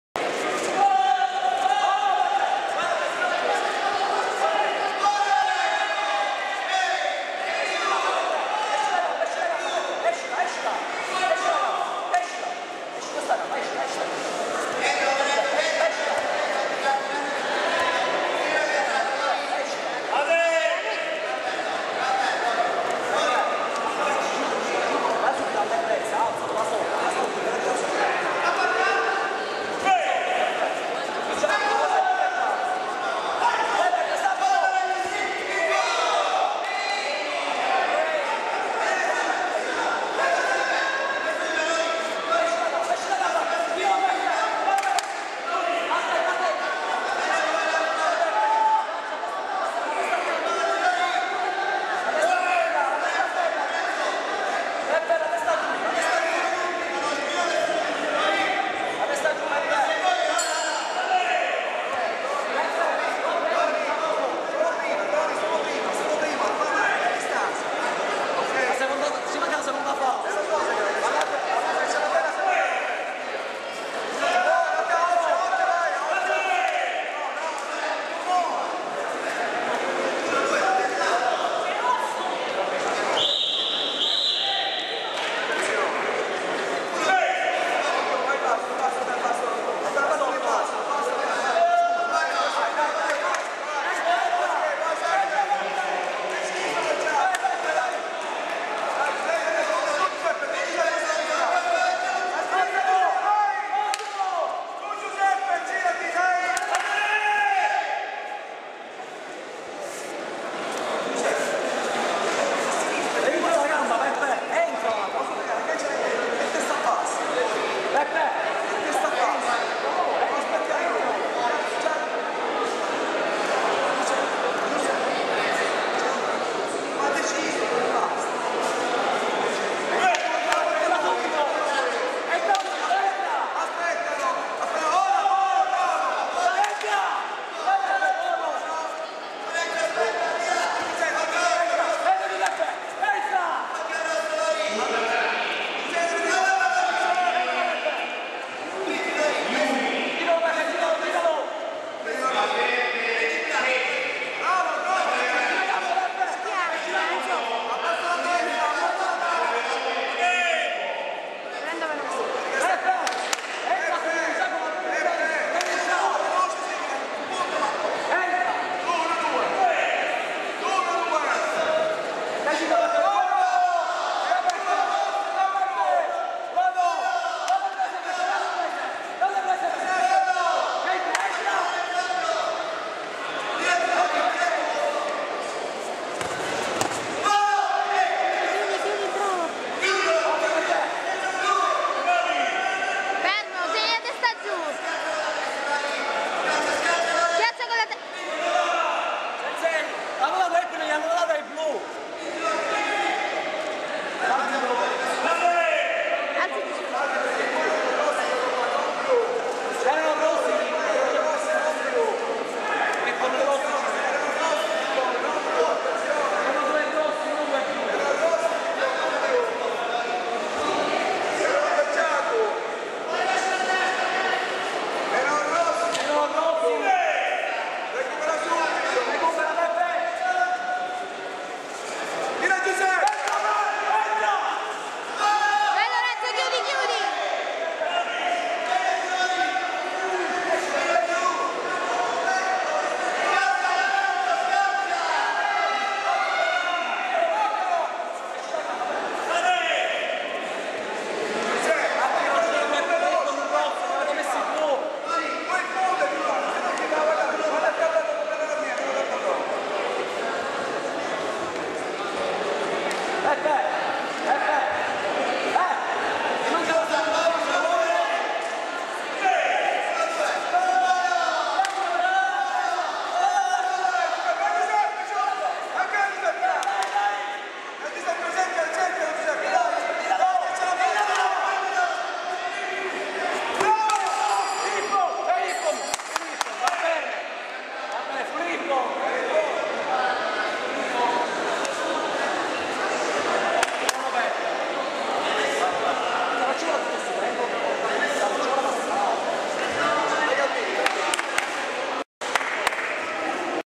coppa italia csen 06_12_09
Coppa Italia C.S.E.N cat -85 kg seniores 1 fascia
nonostante alla fine ho perso l'incontro per fullippon ho deciso di caricare ugualmente il video
perche in fin dei conti è stato un bel combattimento fino alla fine ^^